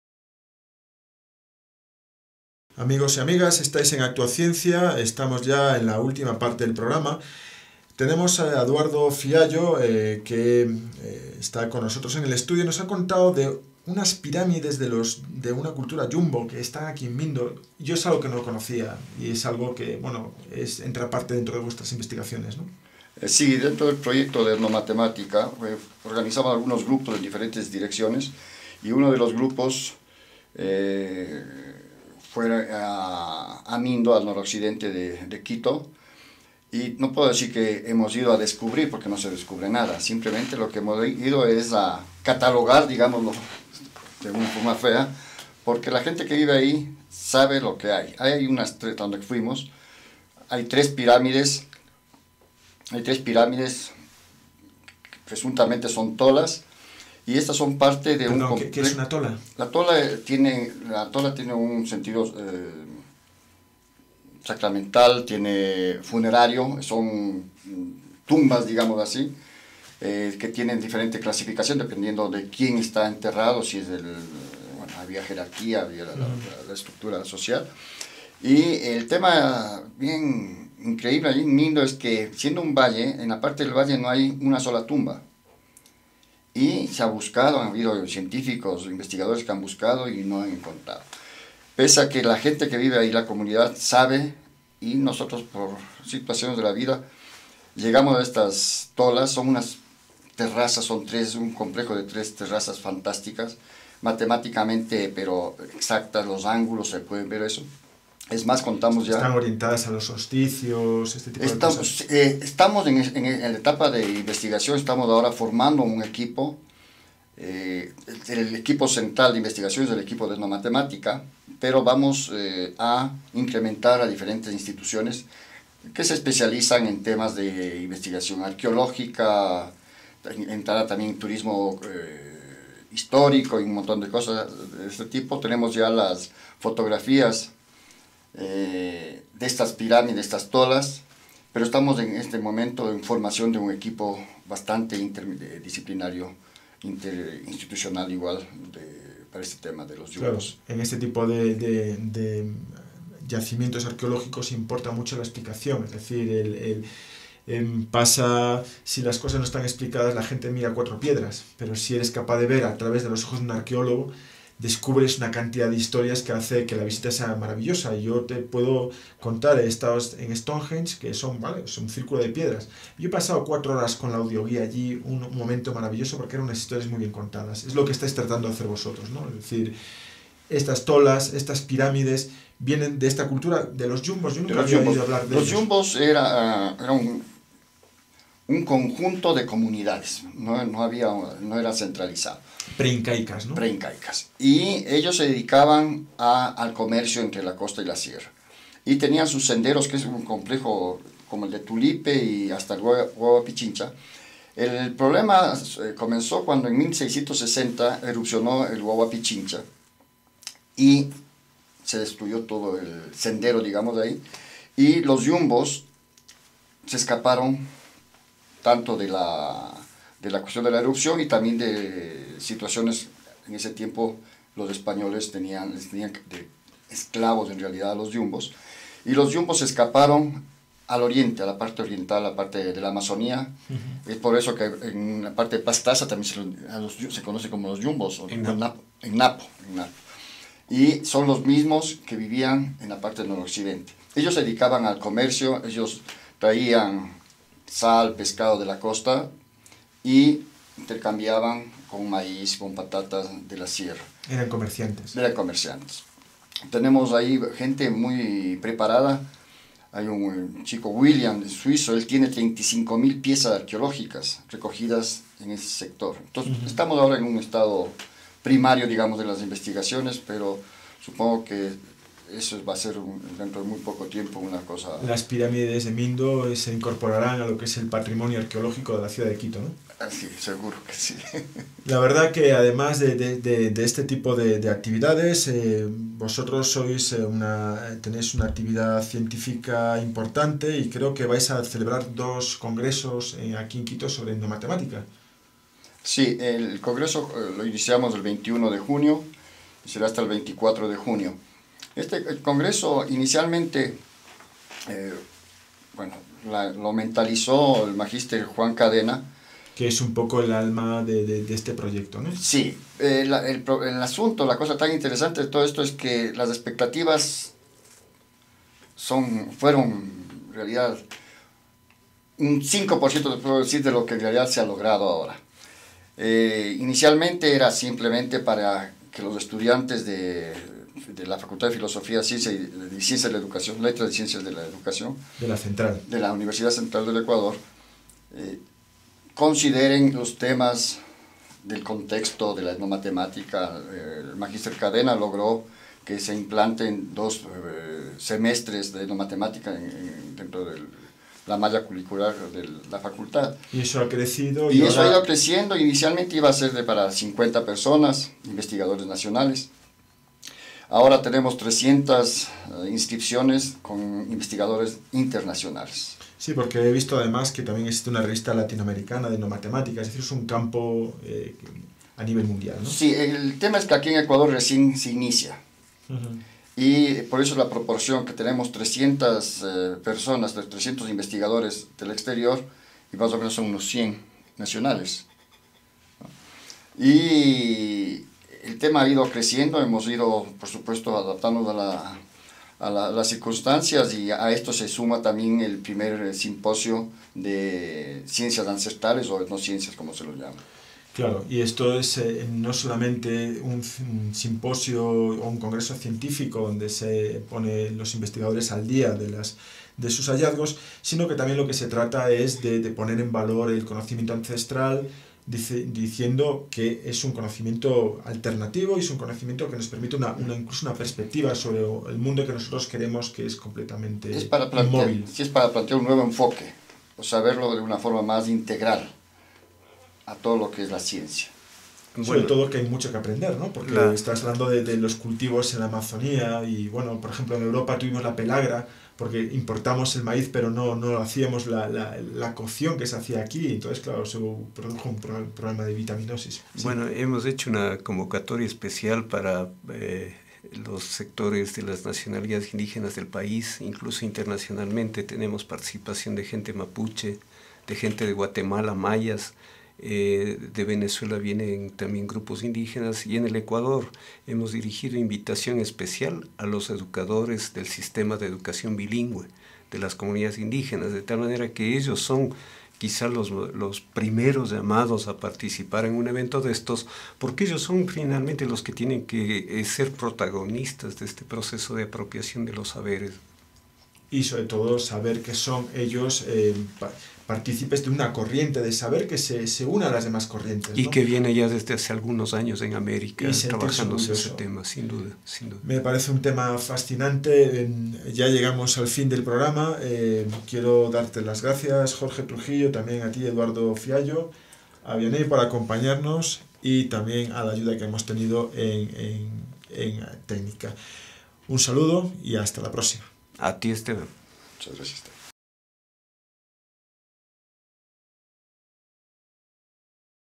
Etnomatemática 5/5
El Proyecto EtnoMATEMÁTICA-RUNAYUPAY del Ecuador
El objetivo general del proyecto es investigar sobre la contradicción entre el ethos  andino y las formas de enseñanza aprendizaje impuestos desde la conquista con miras a presentar una propuesta de incorporación del pensamiento Etnomatemático en la malla curricular de formación de docentes.